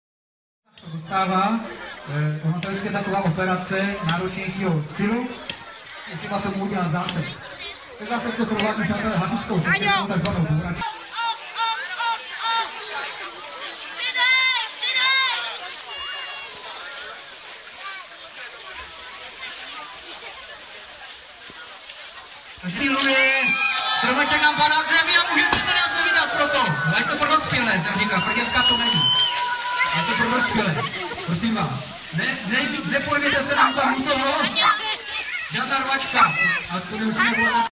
Kácení Májky
Kácení Májky v Hodoňovicích u Frýdku - Místku - Autor: Robert Poledník